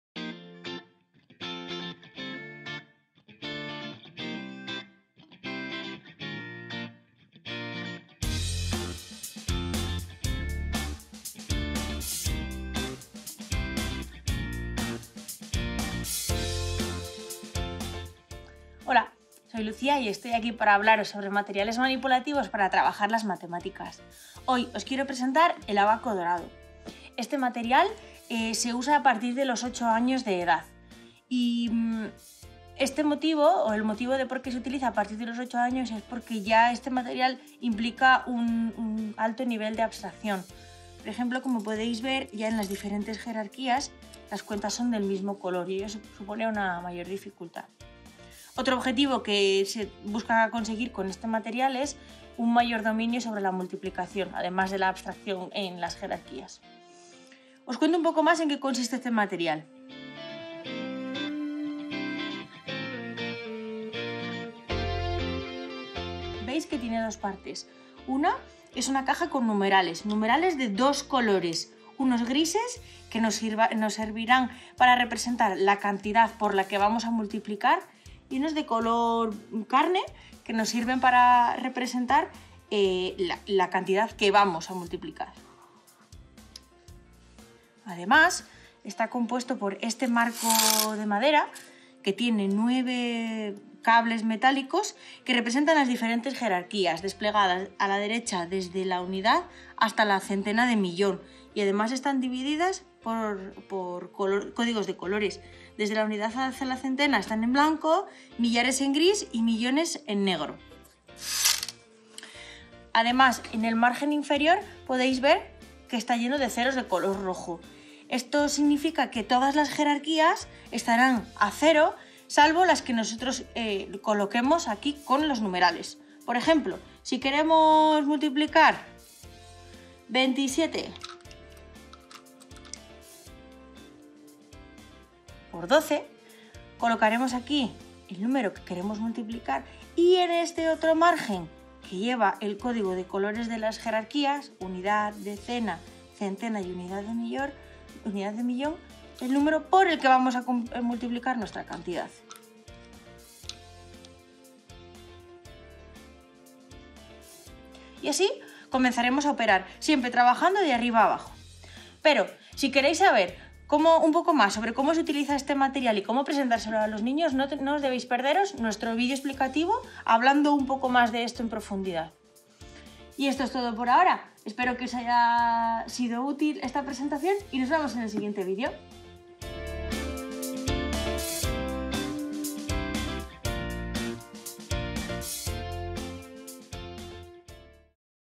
15. Golden Abacus- Materials (EN subtitles) Math-Ools interactive book
Interactive book of the Erasmus+ project: "Math Methodologies and tools for elementary school" KA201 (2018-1-NO01-KA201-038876). Part 1: Materials.